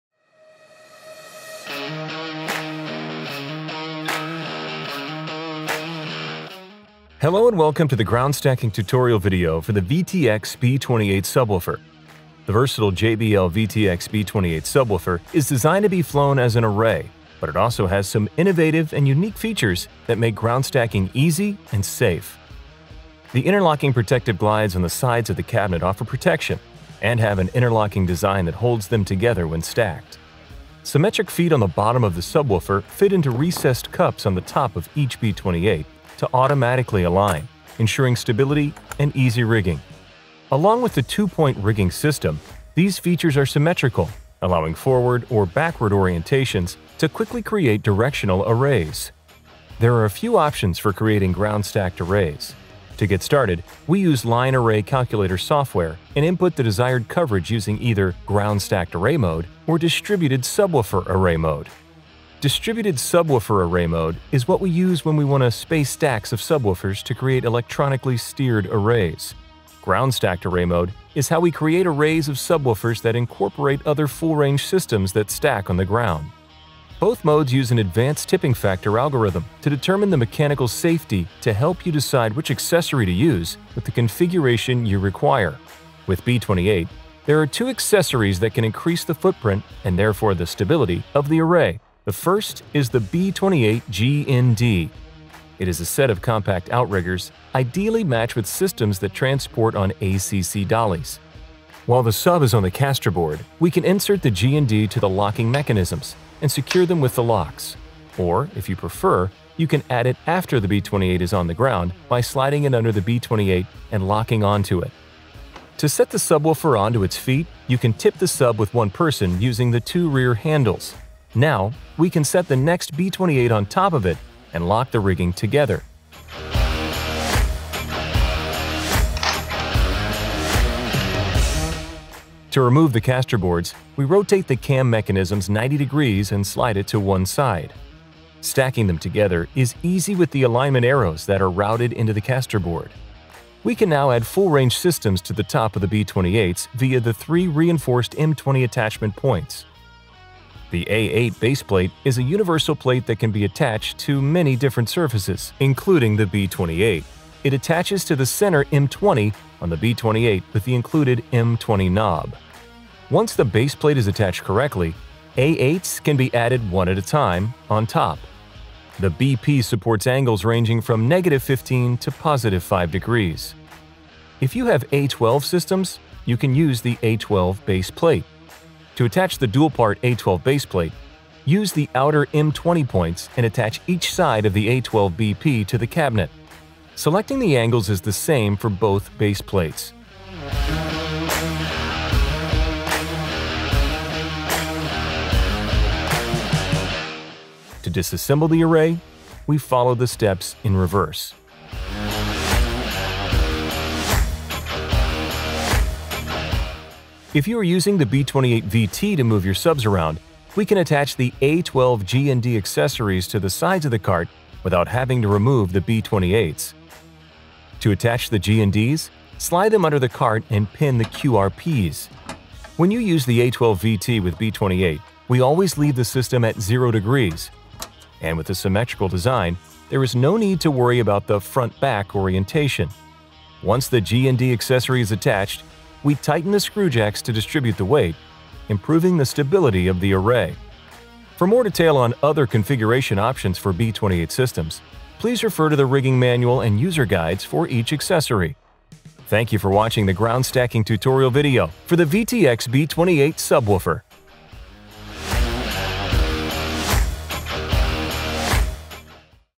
JBL VTX B28 Subwoofer: Ground Stacking Tutorial
The JBL VTX B28 dual 18-inch subwoofer offers innovative and unique features for safely ground stacking your enclosures. From simple setup and customization to using JBL’s Line Array Calculator to optimize your system, see how easy it is to set up and tear down your B28 ground stacks.